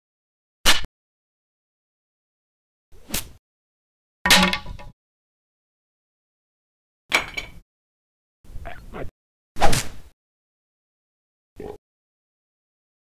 [ANIMATION] I don't know what to call this, stick-mation
My first stick animation in gimp. all foley done myself.